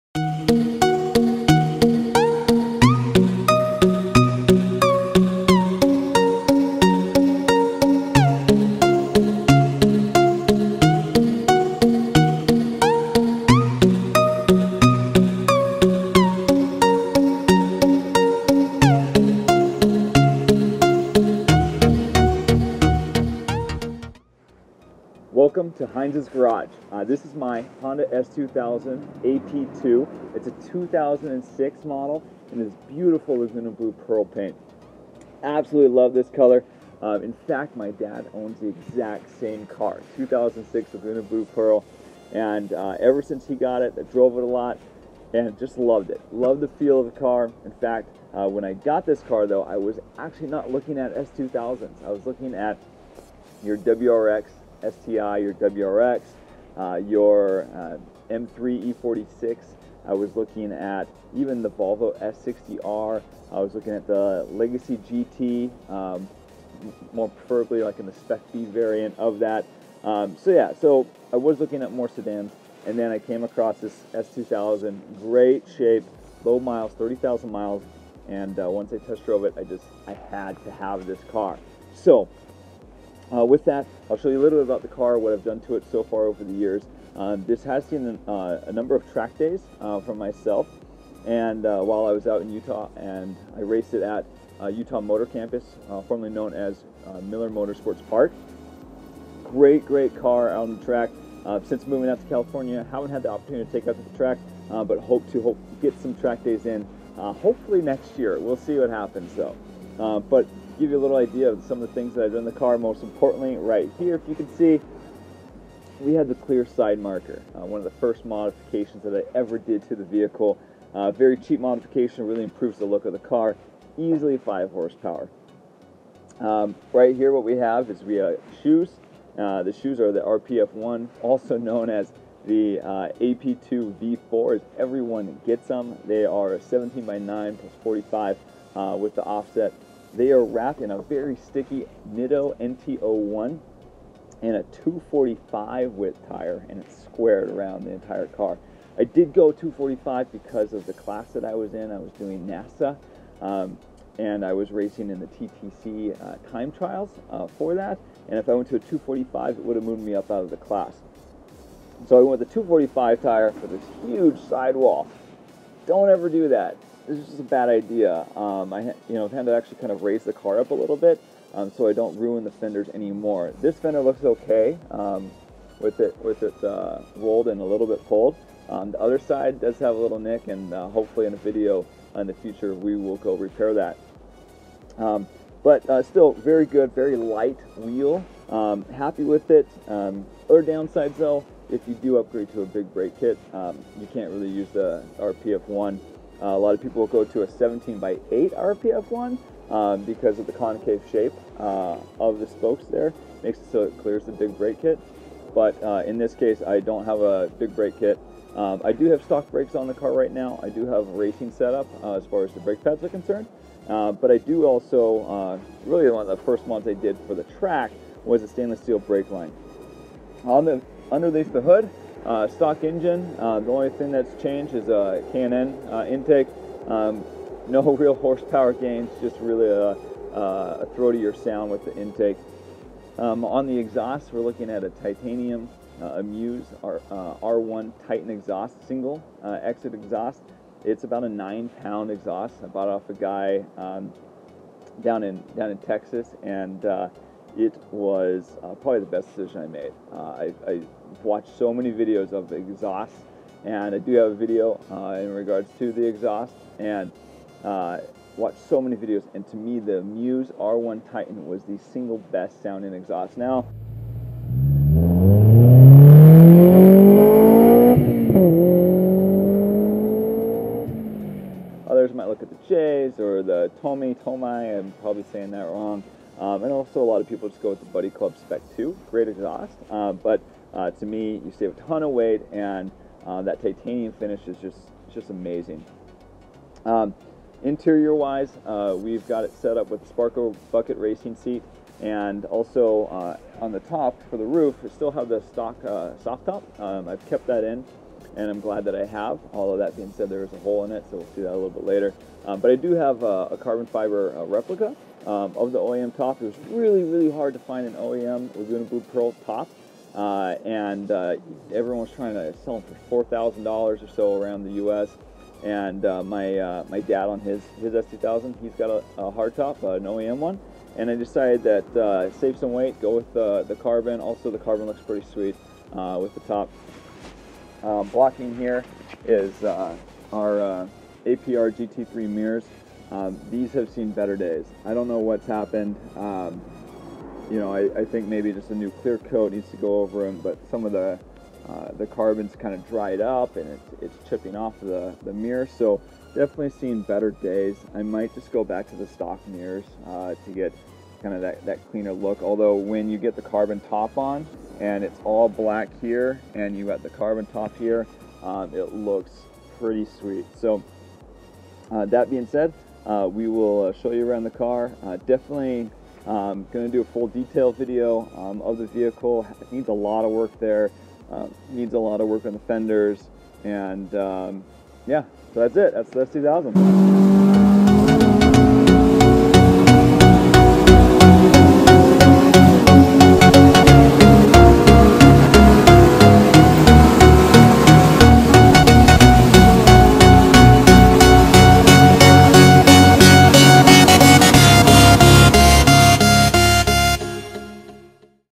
My first sports car: Honda S2000
Intro video of my 2006 Honda S2000. Wuick overview of the work I've done to the care. This is the start of many videos documenting any work I do on the car.